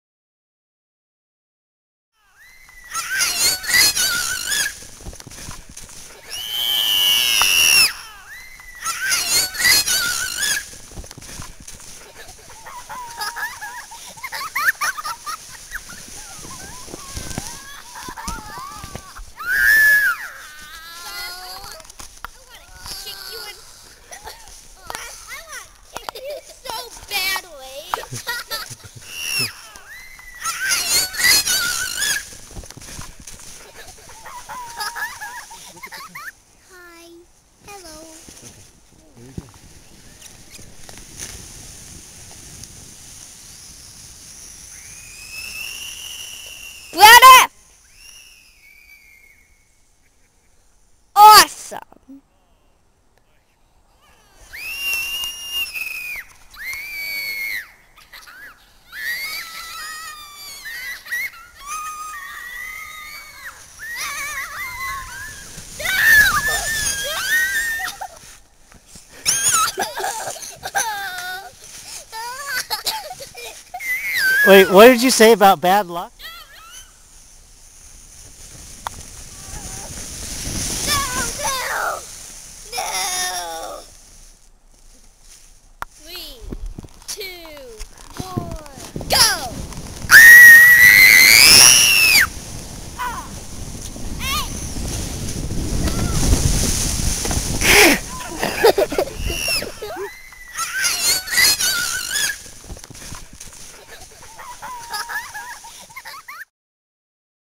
Backyard snow tubing after record snowfall Jan 2011.mp4
E + B + J snow tubing Jan 2011 after record East Coast snowfall.